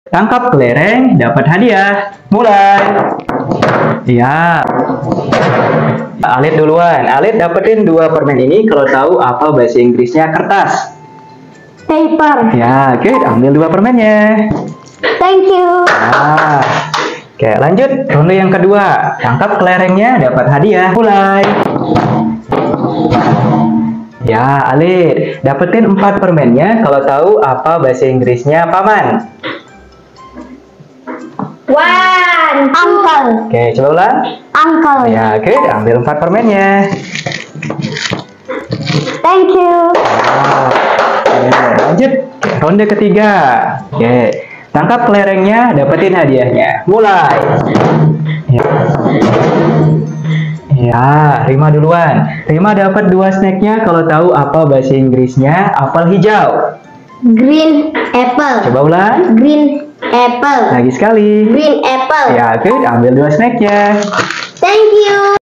Tangkap Kelereng Berhadiah | Alit VS Rima
Seru-seruan belajar kosakata bahasa inggris dalam permainan tangkap kelereng dapat hadiah bareng Alit dan Rima.